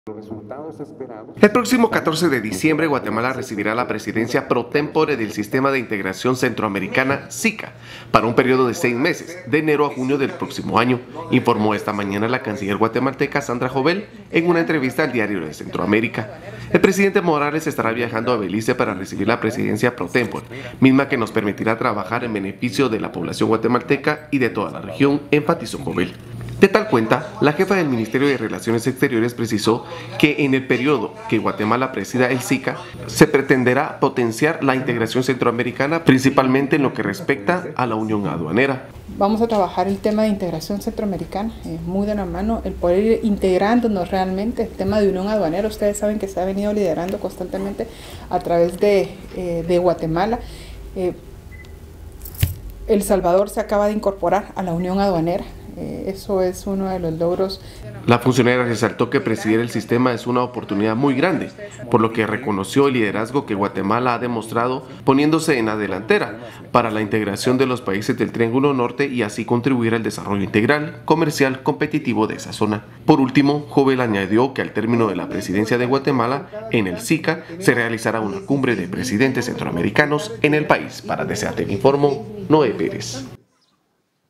Guatemala recibirá en diciembre la Presidencia Pro Témpore del SICA
El próximo 14 de diciembre Guatemala recibirá la Presidencia Pro Témpore del Sistema de Integración Centroamericana (SICA), para un periodo de seis meses (enero a junio de 2019, informó esta mañana la canciller guatemalteca Sandra Jovel, en una entrevista al Diario de Centro América.
“El presidente Morales estará viajando a Belice para recibir la presidencia Pro Témpore, misma que nos permitirá trabajar en beneficio de la población guatemalteca y de toda la región”, enfatizó Jovel.
De tal cuenta, la jefa del Ministerio de Relaciones Exteriores precisó que en el período que Guatemala presida el SICA, se pretenderá “potenciar la Integración Centroamericana, principalmente en lo que respecta a la Unión Aduanera”.
La funcionaria resaltó que presidir el Sistema es una “oportunidad muy grande”, por lo que reconoció “el liderazgo” que ha demostrado Guatemala “poniéndose en la delantera” para la integración de los países del Triangulo Norte, y así contribuir al desarrollo integral, comercial y competitivo de esta zona.
Por último, Jovel añadió que al término de la Presidencia de Guatemala ante el SICA se realizará una cumbre de presidentes centroamericanos en el país